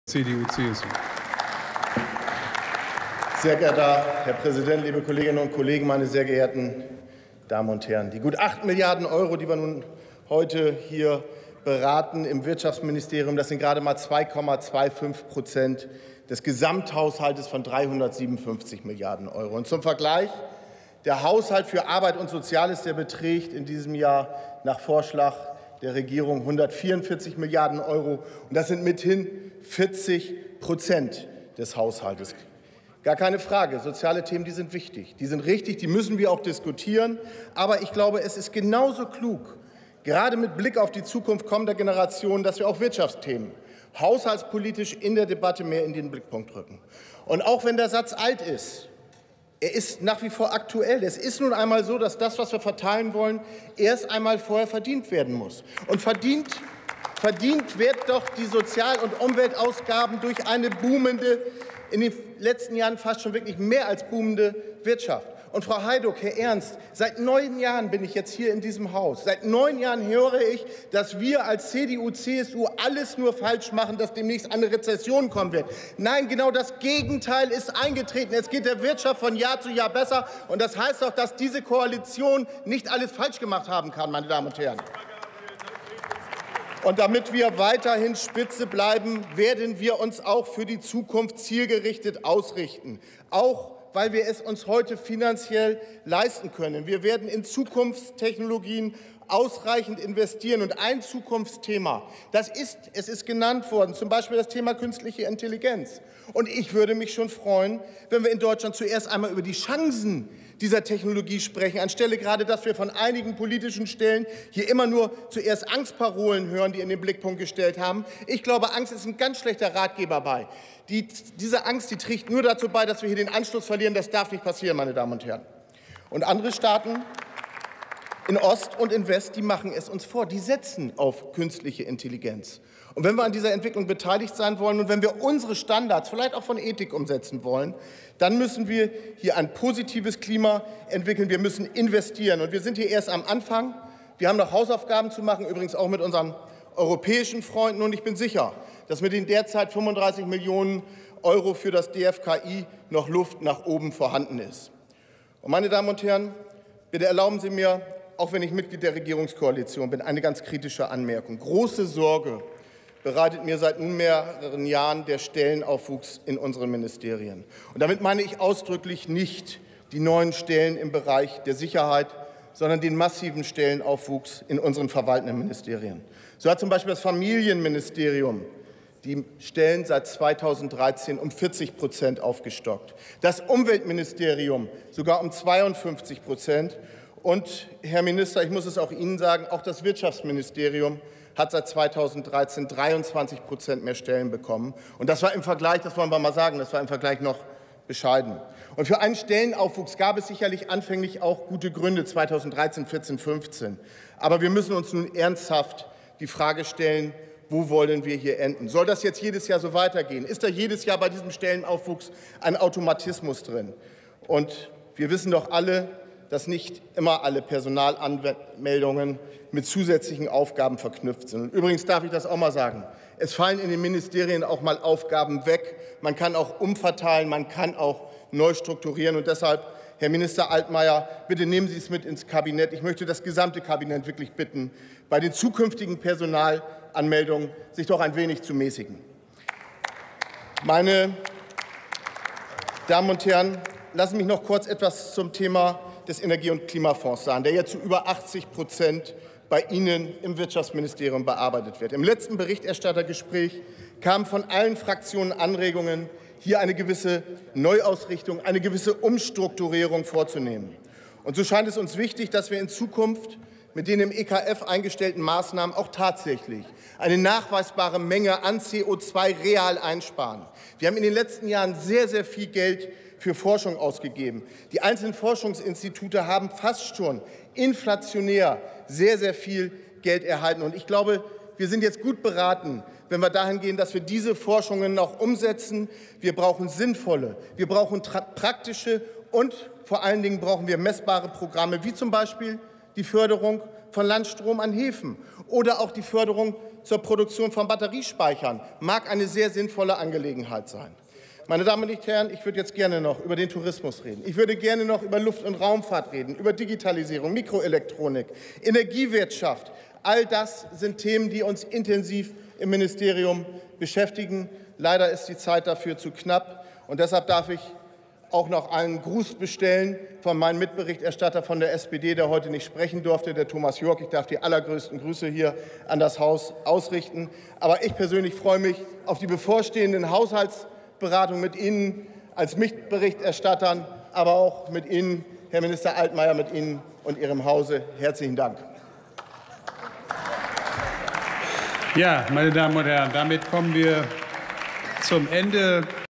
Andreas Mattfeldt: Wirtschaft und Energie [Bundestag 13.09.2018]
Erste Beratung der Bundesregierung
Einzelplan 09 (Wirtschaft und Energie)

19. Deutscher Bundestag
49. Plenarsitzung
Tagesordnungspunkt: Wirtschaft und Energie
Redner: Mattfeldt, Andreas, CDU/CSU
Partei: Christlich Demokratische Union (CDU)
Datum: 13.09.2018
Plenarprotokoll:
UVID:7271412